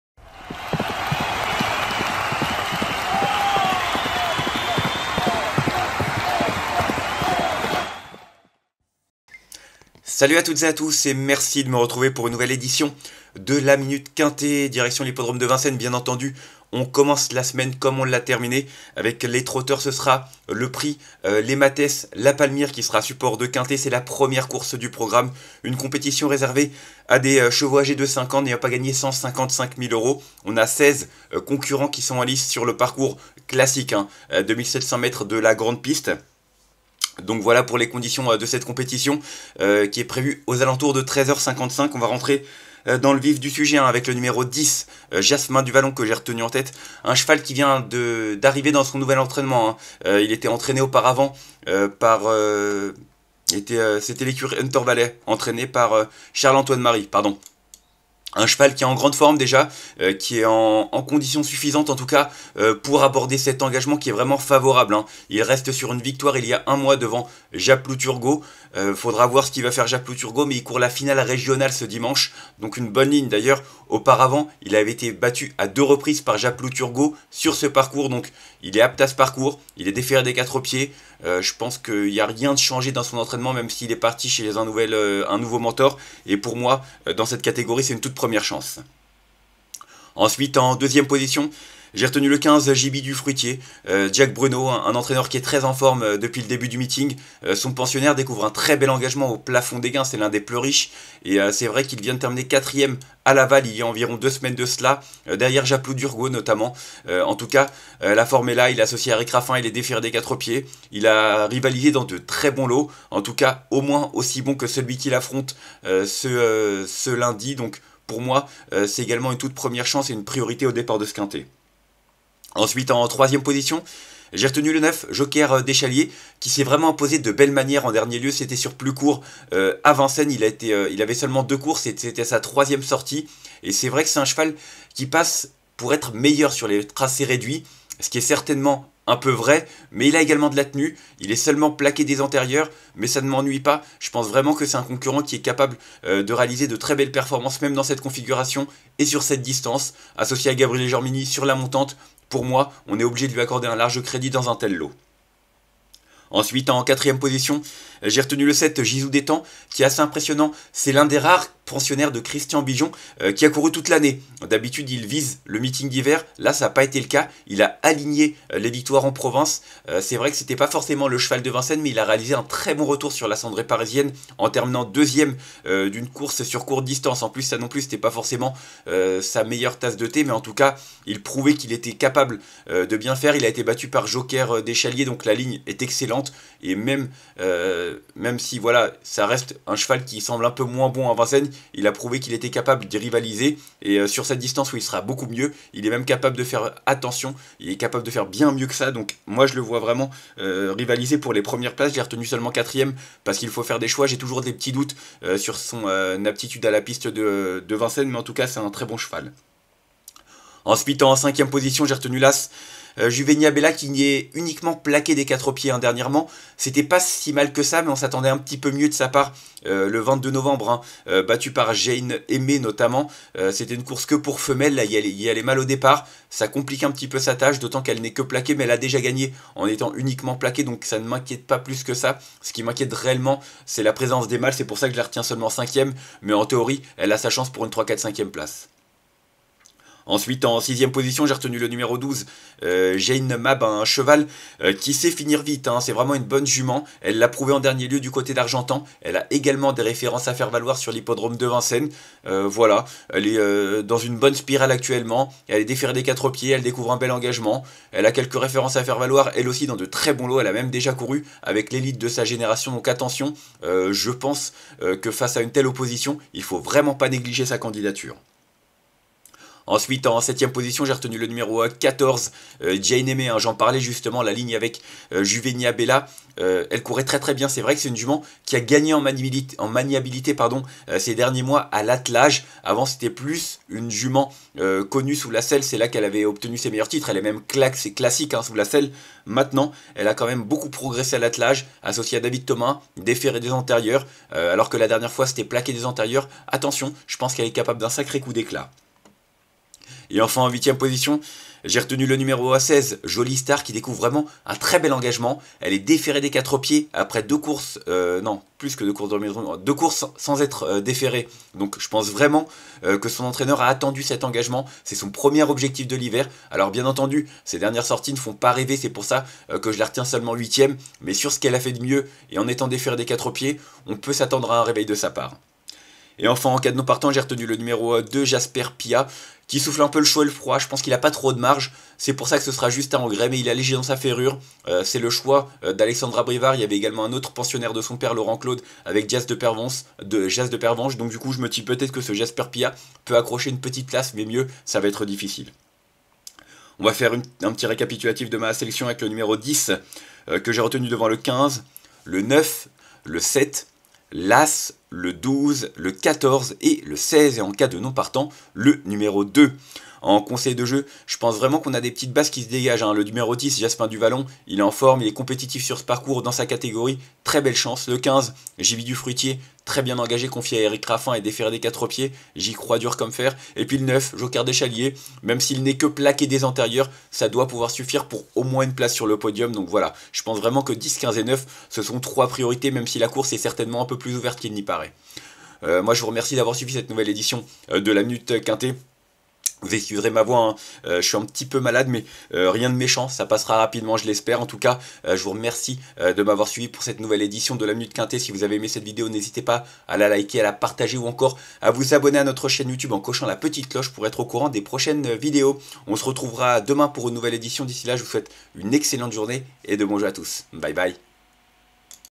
Pronostic Quinté du jour PMU lundi 2 décembre 2024 à Vincennes #341864
Une compétition pour 5 ans d'un bon niveau avec seize concurrents en lice sur 2700 mètres. C'est plus ouvert qu'il n'y paraît, mais de bonnes bases semblent se dégager. Indice de confiance: 3/5.